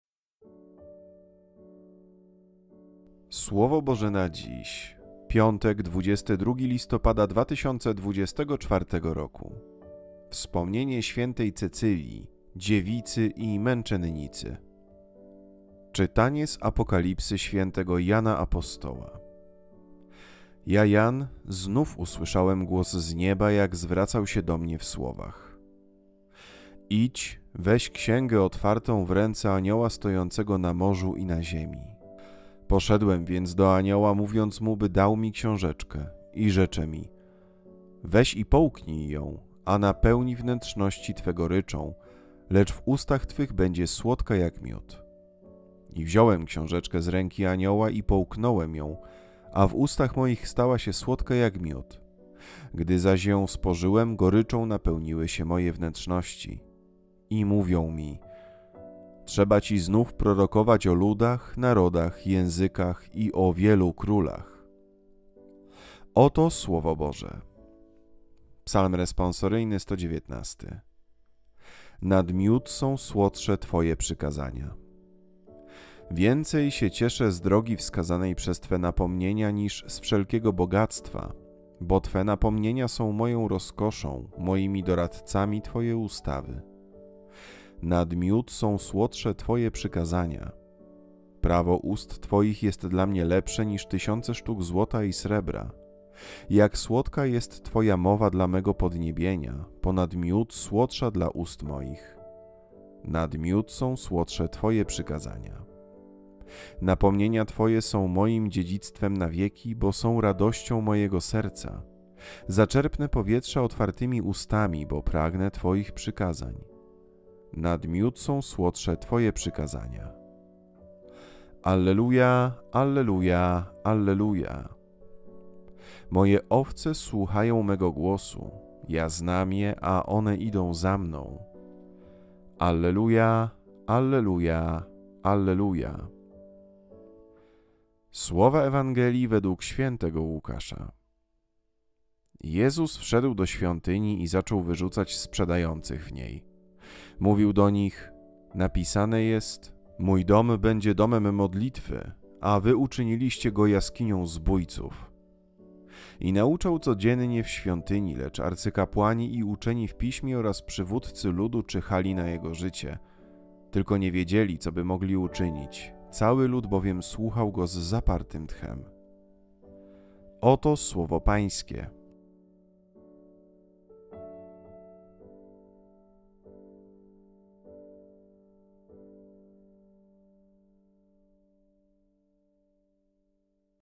Słowo Boże na dziś 22.11.2024r. - Wspomnienie św. Cecylii, dziewicy i męczennicy
WSPOMNIENIE ŚW. CECYLII, DZIEWICY I MĘCZENNICY (czytania z dnia)
PIĄTEK XXXIII TYGODNIA ZWYKŁEGO, ROK II

PIERWSZE CZYTANIE (Ap 10, 8-11)

Książeczka słodka jak zwycięstwa Kościoła i gorzka jak jego doświadczenia

Czytanie z Apokalipsy Świętego Jana Apostoła

Ja, Jan, znów usłyszałem głos z nieba, jak zwracał się do mnie w słowach: «Idź, weź księgę otwartą w ręce anioła stojącego na morzu i na ziemi!»
Poszedłem więc do anioła, mówiąc mu, by dał mi książeczkę. I rzecze mi: «Weź i połknij ją, a napełni wnętrzności twe goryczą, lecz w ustach twych będzie słodka jak miód». I wziąłem książeczkę z ręki anioła, i połknąłem ją, a w ustach moich stała się słodka jak miód, gdy zaś ją spożyłem, goryczą napełniły się moje wnętrzności.
I mówią mi: «Trzeba ci znów prorokować o ludach, narodach, językach i o wielu królach».

PSALM RESPONSORYJNY (Ps 119 (118), 14 i 24. 72 i 103. 111 i 131)

Refren: Nad miód są słodsze Twoje przykazania.

Więcej się cieszę z drogi wskazanej przez Twe napomnienia *
niż z wszelkiego bogactwa.
Bo Twe napomnienia są moją rozkoszą, *
moimi doradcami Twoje ustawy.

Prawo ust Twoich jest dla mnie lepsze *
niż tysiące sztuk złota i srebra.
Jak słodka jest Twoja mowa dla mego podniebienia, *
ponad miód słodsza dla ust moich.

Napomnienia Twoje są moim dziedzictwem na wieki, *
bo są radością mojego serca.
Zaczerpnę powietrza otwartymi ustami, *
bo pragnę Twoich przykazań.

ŚPIEW PRZED EWANGELIĄ (Por. J 10, 27)

Moje owce słuchają mego głosu,
Ja znam je, a one idą za Mną.

Jezus wypędza przekupniów ze świątyni

Jezus wszedł do świątyni i zaczął wyrzucać sprzedających w niej. Mówił do nich: «Napisane jest: „Mój dom będzie domem modlitwy”, a wy uczyniliście go jaskinią zbójców».
I nauczał codziennie w świątyni. Lecz arcykapłani i uczeni w Piśmie oraz przywódcy ludu czyhali na Jego życie. Tylko nie wiedzieli, co by mogli uczynić, cały lud bowiem słuchał Go z zapartym tchem.

Od niedawna istnieje możliwość wsparcia kanału. Z serca dziękuję, jeśli się zdecydujesz wesprzeć jego rozwój, więcej info pod poniższym linkiem: